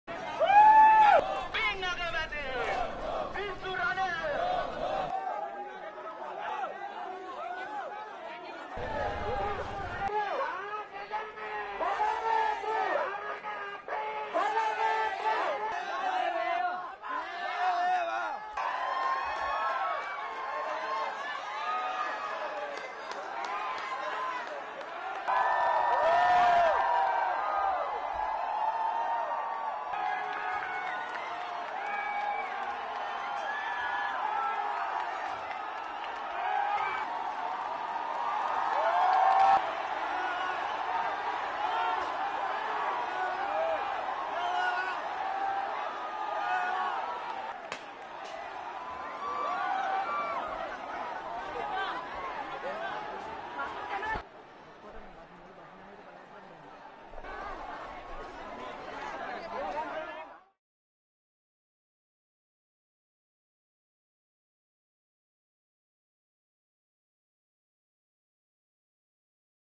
Sri Lanka Crisis: state of emergency declared | Protesters storm prime minister's office
Sri Lankan protesters breached the walls of Prime Minister Ranil Wickremesinghe's office in the capital Colombo on Wednesday (July 13) as the government declared a state of emergency.

Hundreds of protesters, marching right past rows of riot police, surrounded the prime minister's office at around noon (0630 GMT), demanding his immediate resignation.

Riot police fired several rounds of tear gas as protesters climbed over the compound’s fences.

Sri Lankan protesters storm prime minister's office as state of emergency declared
Tear gas fired at protesters outside Sri Lankan PM's office